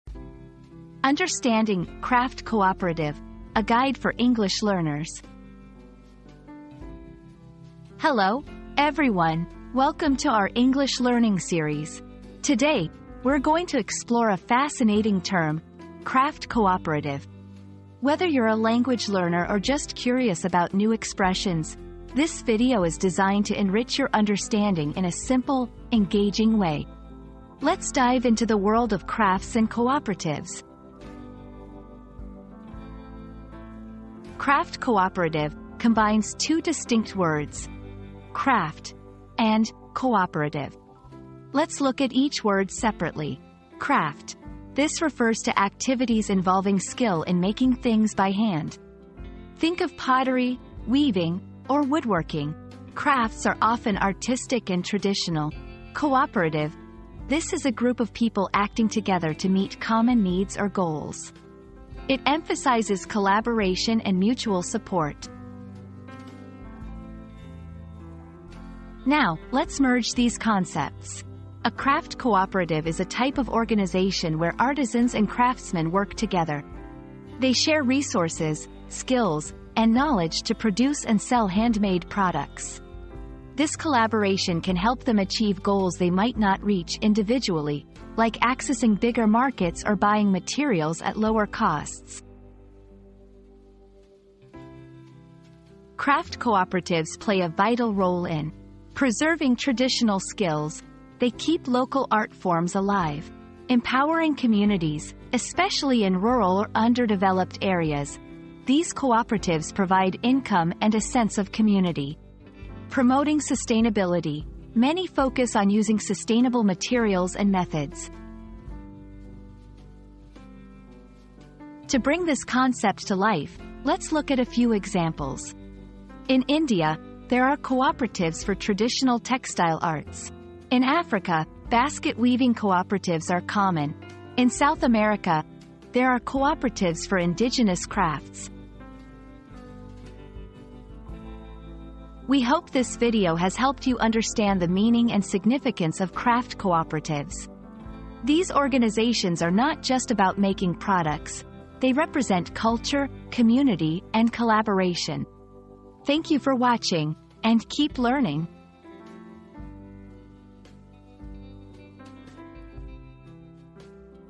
Understanding "Craft Cooperative": A Guide for English Learners
00:00 • Introduction - Understanding "Craft Cooperative": A Guide for English Learners
00:33 • Breaking Down the Term
01:08 • What is a Craft Cooperative?
01:35 • Why are Craft Cooperatives Important?
02:01 • Examples Around the World